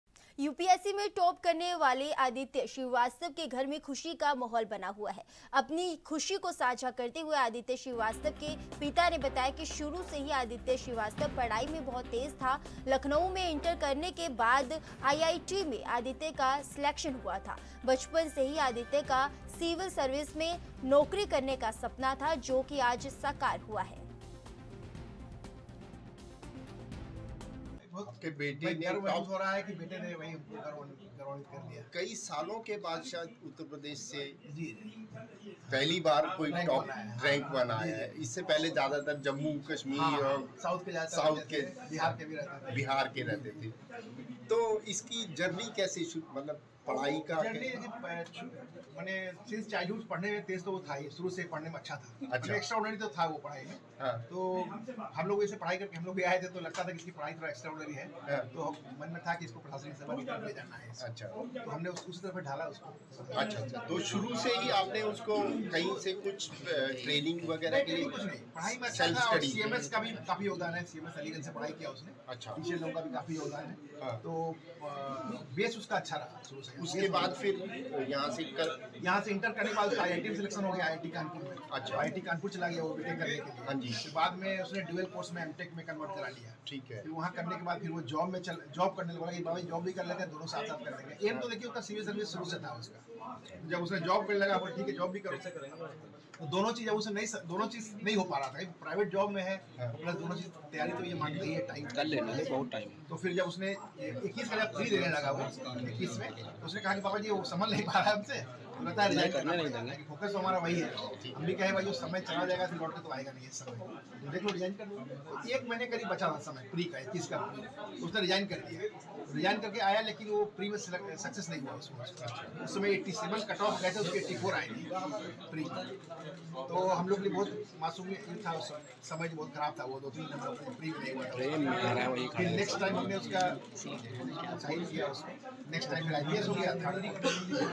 यूपी: UPSC में टॉप करने वाले आदित्य के घर में खुशी का माहौल| ANB NEWS
लखनऊ , यूपी
UPSC में टॉप करने वाले आदित्य के घर में खुशी का माहौल
आदित्य श्रीवास्तव के माता-पिता इस कामयाबी से हुए बहुत खुश
आज मेरे बेटे का सपना हुआ सरकार- आदित्य के पिता